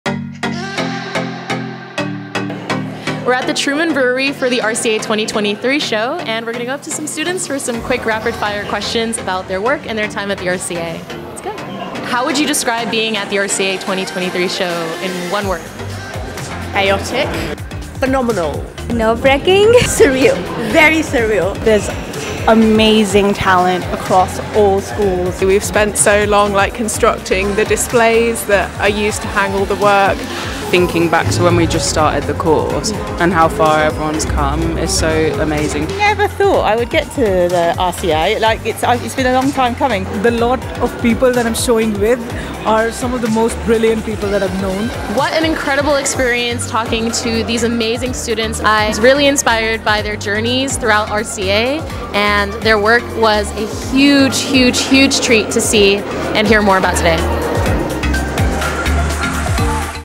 How would you describe RCA2023 Graduate Show in a word?
"Phenomenal" "Surreal" "VERY surreal!"

Cyan D'Anjou, an IED student at the RCA, chatted with fellow students during the RCA2023 Graduate Show at Truman Brewery, delving into their remarkable experiences at the College and their incredible showcases.

Don't miss the opportunity to witness this extraordinary event firsthand!

We're open from 12 - 6pm. The last entry is at 5pm, so we encourage you to arrive early to secure your entry. See you there!